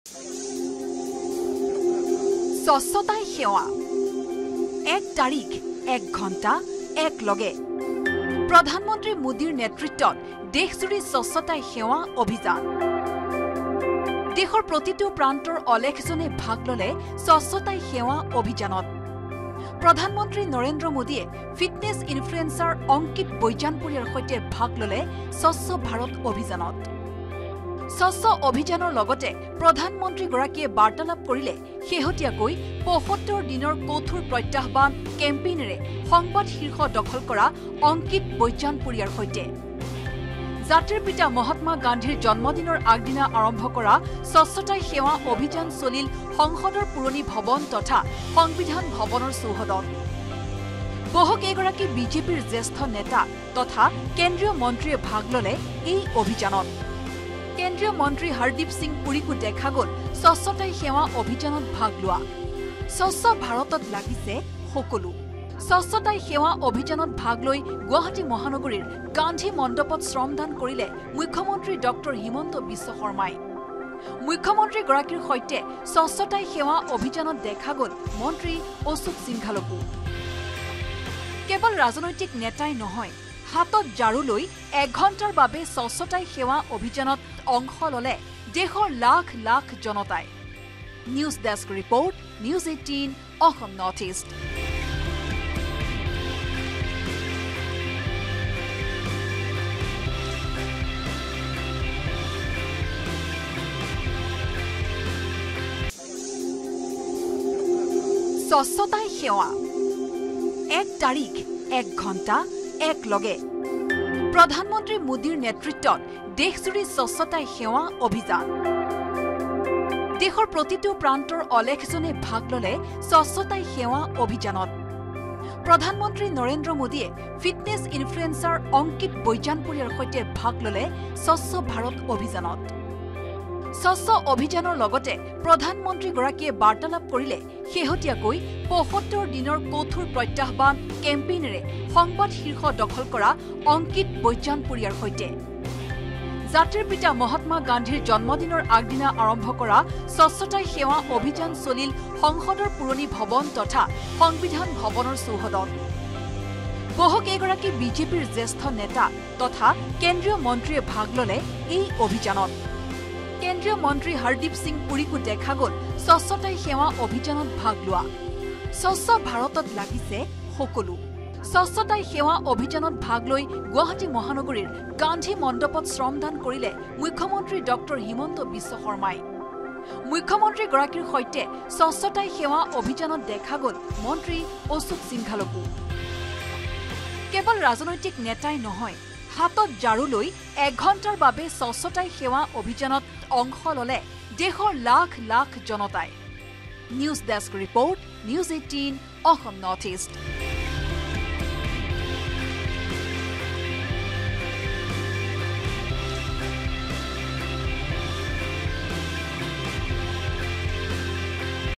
Swachhata Hi Seva : দেশৰ প্ৰান্তে প্ৰান্তে চলিল স্বচ্ছতাই সেৱা অভিযান | PM Modi | N18V
Swachhata Hi Seva | প্ৰধানমন্ত্ৰী মোদীৰ নেতৃত্বত দেশজুৰি স্বচ্ছতাই সেৱা অভিযান। দেশৰ প্ৰান্তে প্ৰান্তে চলিল স্বচ্ছতাই সেৱা অভিযান। অভিযানত বিজেপি নেতা, মন্ত্ৰীৰ লগতে ভাগ ল’লে বহু লোকে।